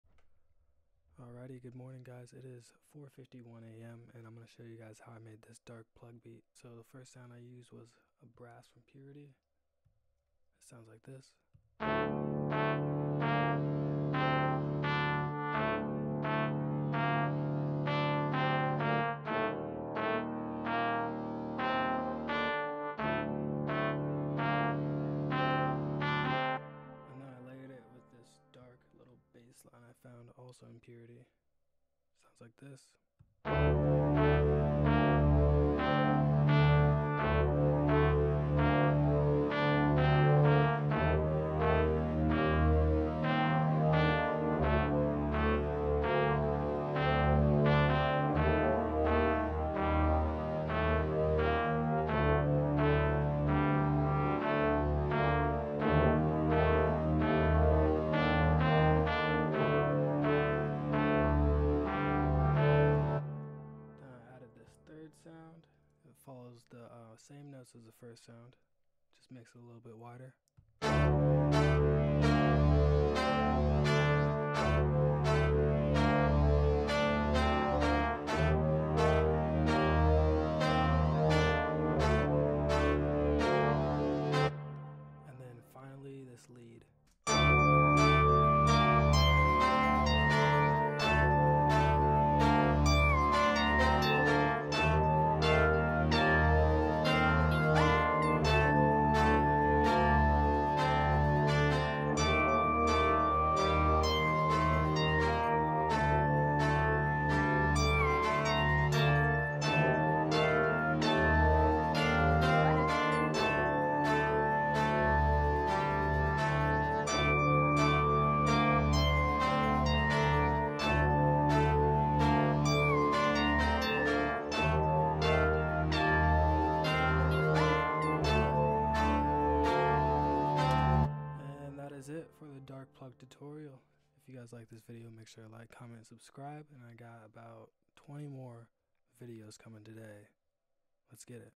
Dark Plugg 101: A Beginner's Guide
in this video , i too am a beginner but after i post about 20 more dark plugg videos i will have the credentials to make dark plugg for these rappers and there mother for daysssss i tell you, big truss the process. Hopefully one of these videos does well as the beats get better.